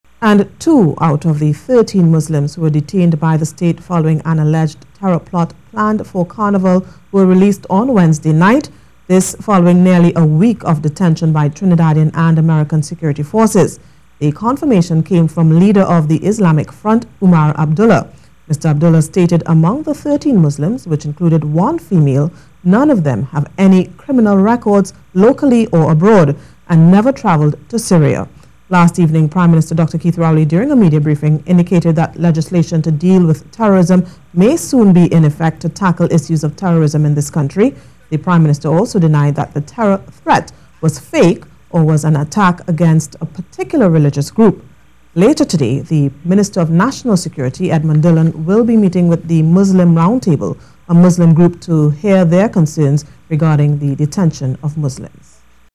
Two Carnival Terror Suspects Released
Two out of the 13 Muslims who were detained by the State, following an alleged terror plot planned for Carnival, were released on Wednesday night.

This, following nearly a week of detention by security forces.

The confirmation came from Leader of the Islamic Front, Umar Abdullah.

Mr. Abdullah stated amongst the 13 Muslims, including one female, none of them had any criminal records locally or abroad, or travelled to Syria.

During a Media Briefing on Wednesday, Prime Minister Dr. Keith Rowley indicated that legislation to deal with terrorism may soon be in effect to tackle issues of terrorism in this country.

The Prime Minister also denied that the terror threat was fake or was an attack against a particular religious group.

The Minister of National Security Edmund Dillon will be meeting with the Muslim Roundtable to hear their concerns regarding the detention of Muslims.